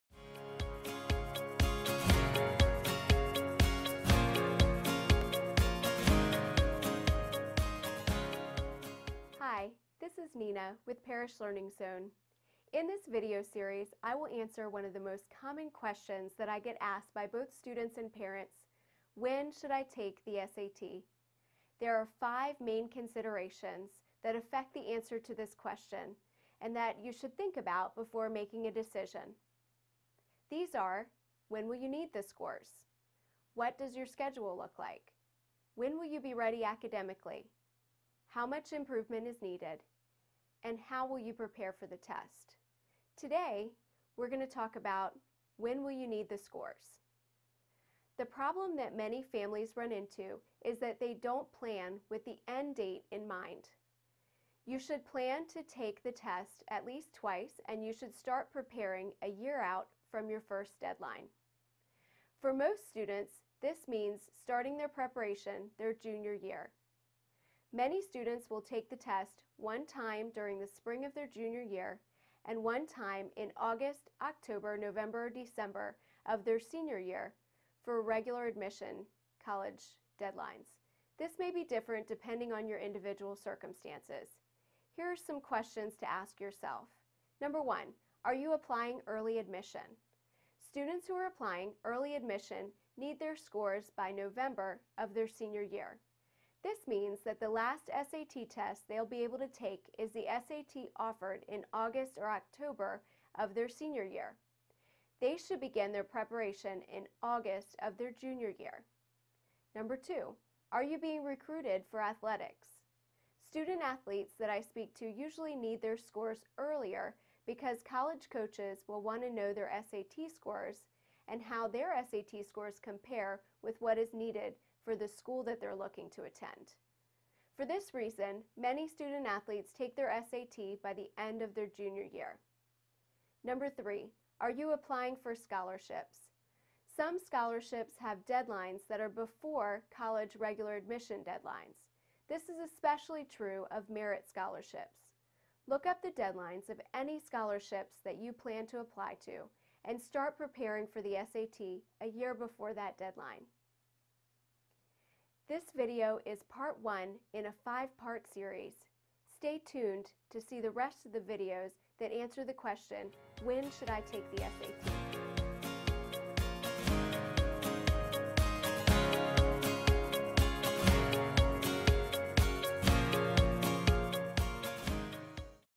When Should I Take The SAT? Part 1 of 5
In this blog, I will answer one of the most common questions that I get asked by both students and parents: “When should I take the SAT?” There are five main factors that affect the answer to this question and that you should consider before making a decision:

When will you need the scores; What does your schedule look like; When will you be ready academically; How much improvement is needed; and How will you prepare for the test?

1. When Will You Need the Scores?
The problem that many families run into is that they do not plan with the end-date in mind. You should plan to take the test twice and you should start preparing a year out from your first deadline. For most students this means starting their preparation their Junior year. Many students will take the test one time during the spring of their Junior year and one time in August, October, November or December of their Senior year for regular admission deadlines. This may be different depending on your individual circumstances. Here are some questions to ask:

Are you applying early admission?
Students who are applying early admission need their scores by November of their Senior year. This means that the last SAT test they will be able to take is the SAT offered in August or October of their Senior year. They should begin preparing in August of their Junior year.

Are you being recruited for athletics?
The student-athletes that I speak with usually need their scores earlier because college coaches will want to know SAT scores and how they compare with what is needed for their school when they are looking to recruit you. For this reason, many athletes take the SAT by the end of their junior year.

Are you applying for scholarships?
Some scholarships have deadlines that are before college regular admission deadlines. This is especially true of merit scholarships. Look up the deadlines of any scholarships you plan to apply to and start preparing for the SAT a year before that deadline.